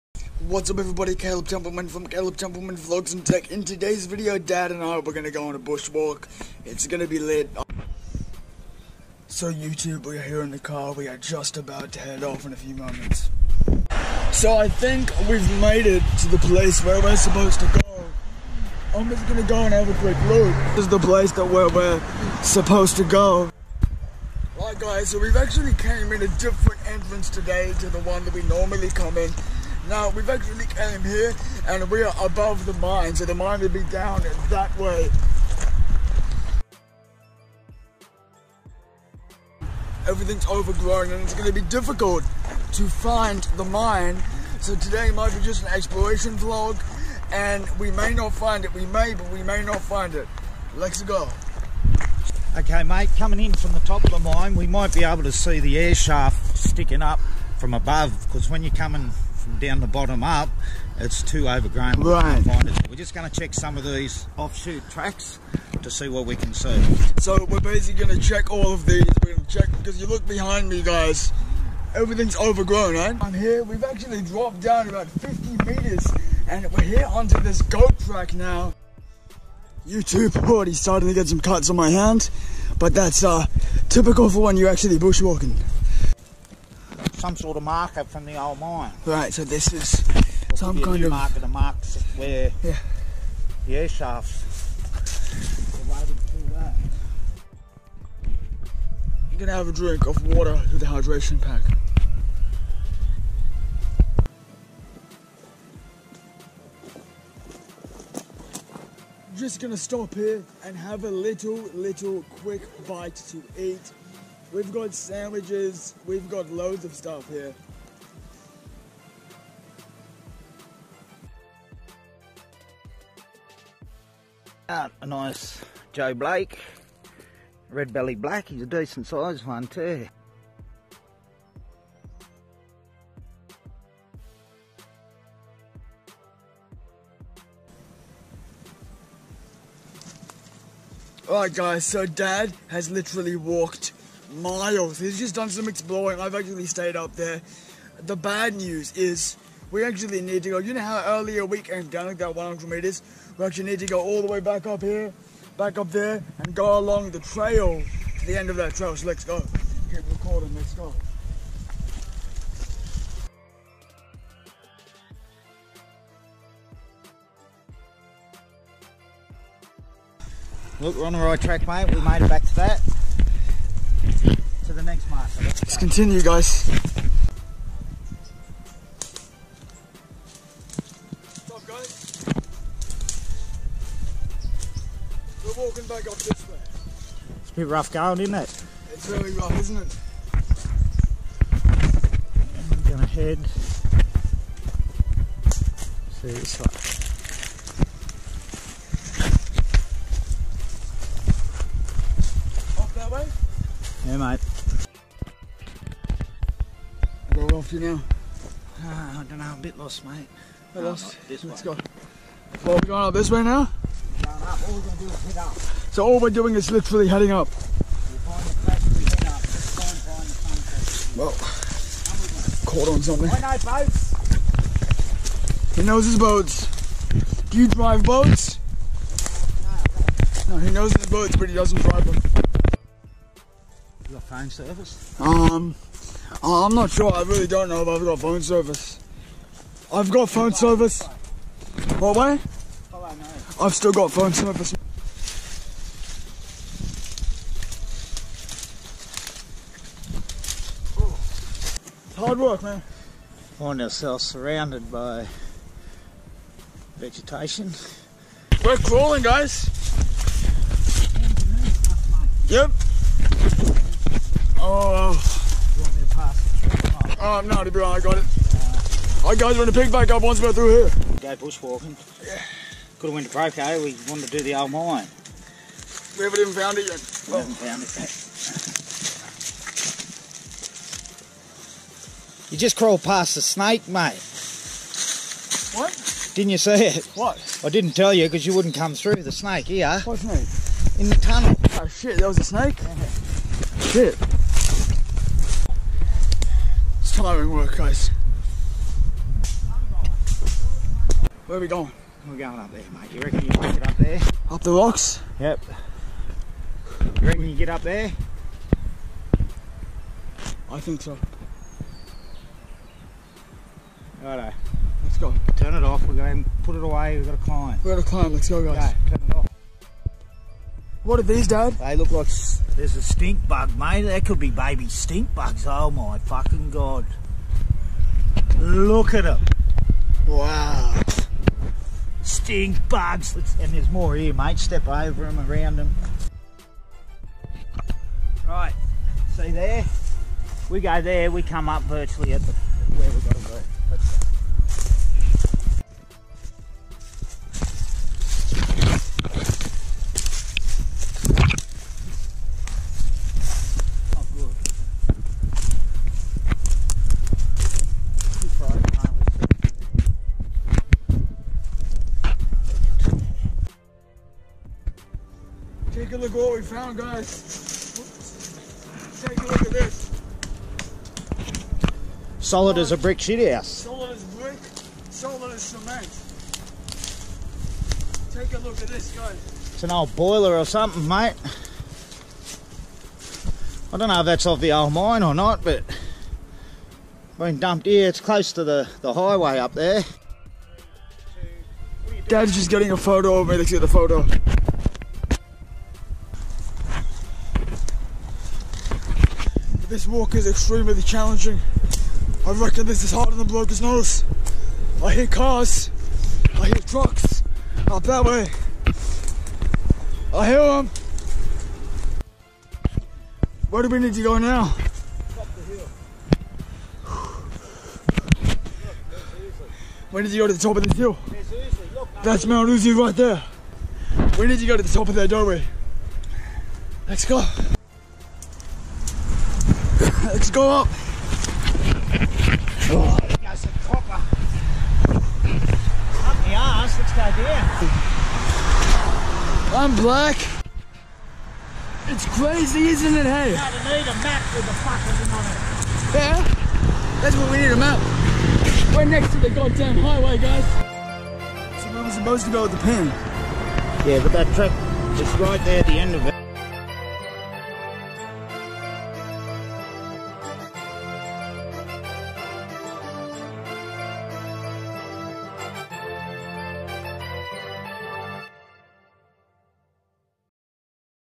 BUSHWALKING VLOG | SNAKES & CLIIFFS, *NEARLY GOT LOST*
Today we go on a bushwalk along the Illawarra Escarpment, we attempt to find the mine, but it all goes down when we see snakes, and we are forced fo climb cliffs and walk kilometres.

Email – kalabhelp(at)gmail(dot)com